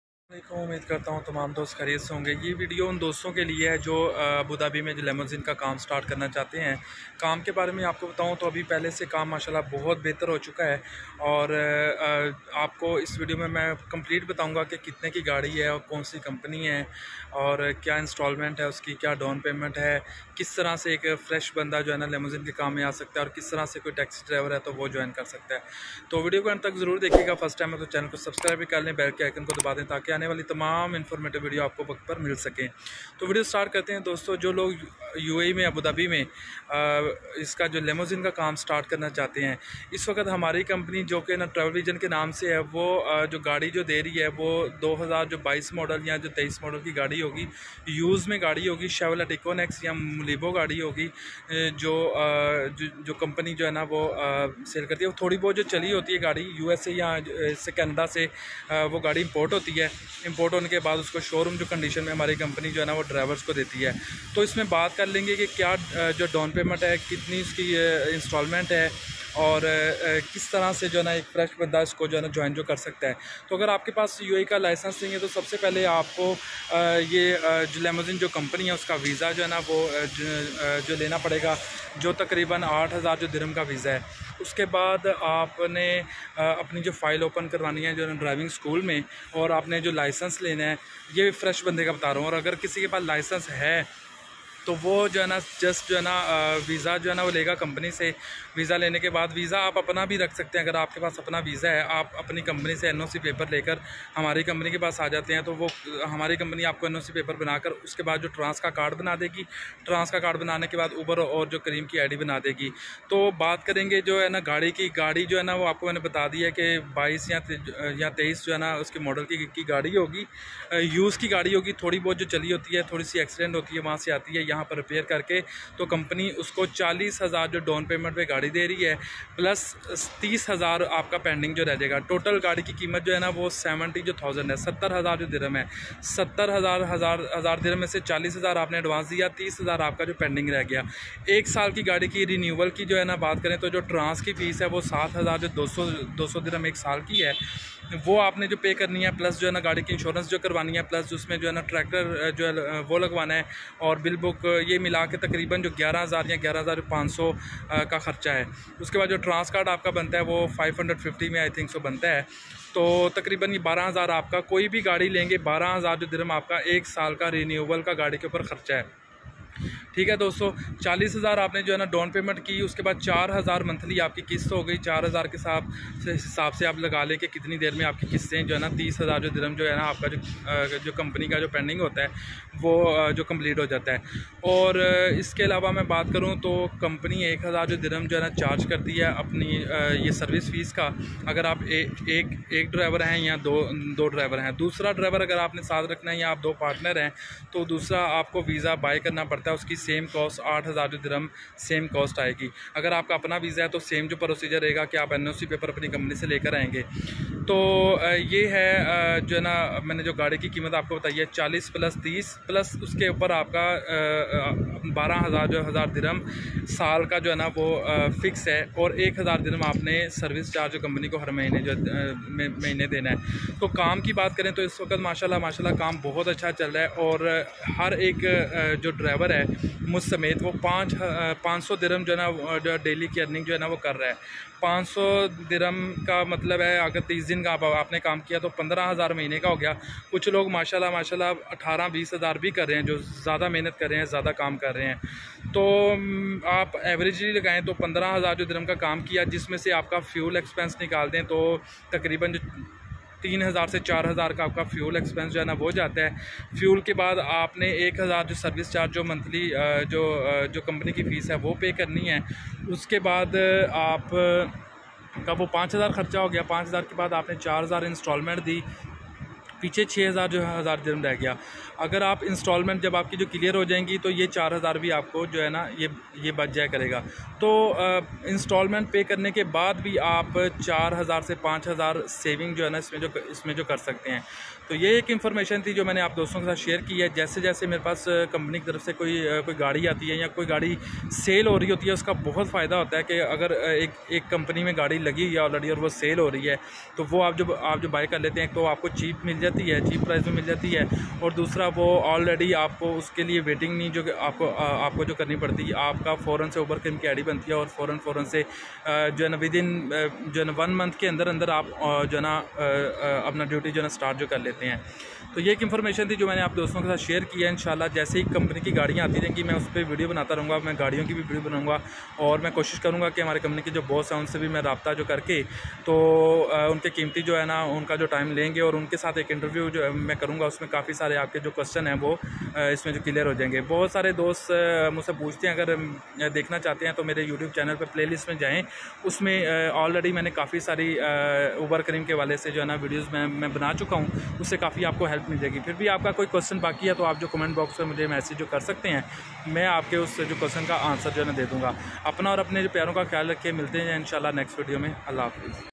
Limousine Business In Dubai Abu Dhabi - Limousine Ka Kam Kesay Start Kre Complete Information
Asslam o Alykum Everyone,

Limousine Work In Dubai Abu Dhabi
Dubai me abi limousine ka kam kesa hai
Uber work in Dubai